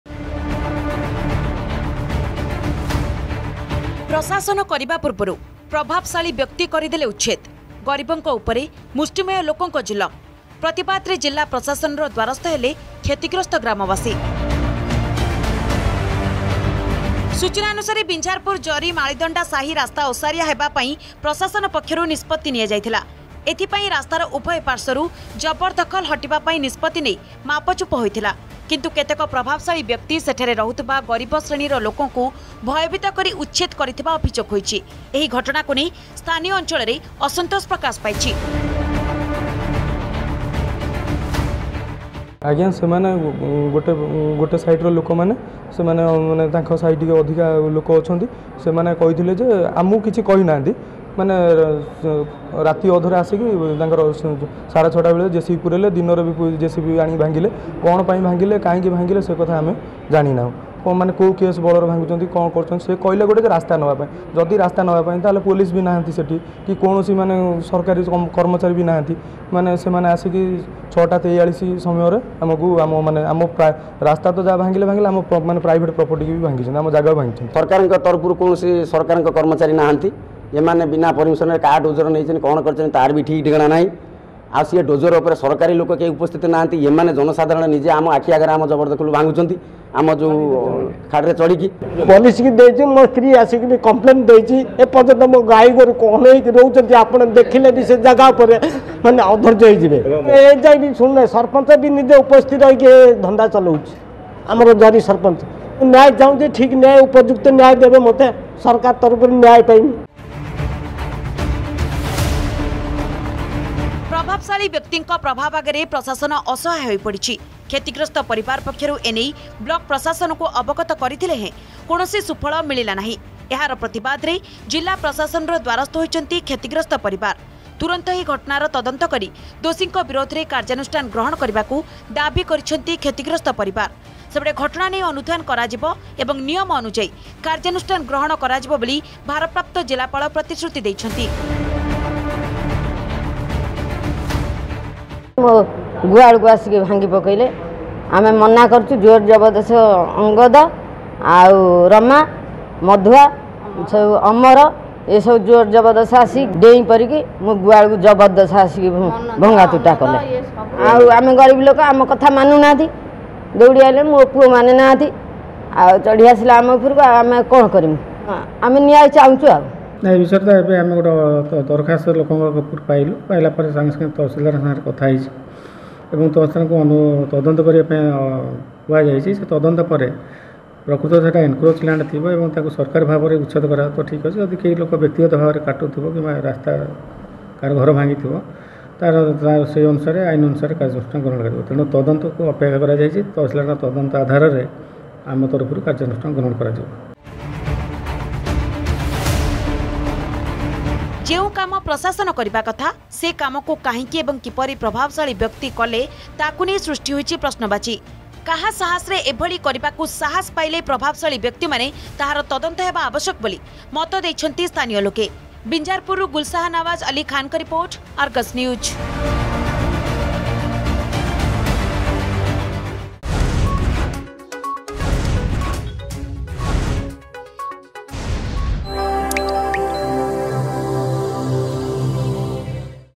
ପ୍ରଶାସନ କରିବା ପୂର୍ବରୁ ପ୍ରଭାବଶାଳୀ ବ୍ୟକ୍ତି କରିଦେଲେ ଉଛେଦ !
ପ୍ରଶାସନ ଜବର ଦଖଲ ଉଛେଦ କରିବା ପୂର୍ବରୁ ମୁଷ୍ଟିମେୟ ଲୋକେ ଗରିବ ଲୋକଙ୍କୁ କଲେ ଉଛେଦ । ଯାଜପୁର ବିଂଝାରପୁର ବ୍ଳକ ଅନ୍ତର୍ଗତ ଜରୀ ପଂଚାୟତ ମାଳିଦଣ୍ଡା ସାହିରେ ଘଟିଛି ଏଭଳି ଘଟଣା । ଯାହାକୁ ନେଇ ଏବେ ଦେଖାଦେଇଛି ବିବାଦ। ଏହାର ପ୍ରତିବାଦ କରି ଜିଲା ପ୍ରଶାସନର ଦ୍ୱାରସ୍ଥ ହୋଇଛନ୍ତି ଗ୍ରାମବାସୀ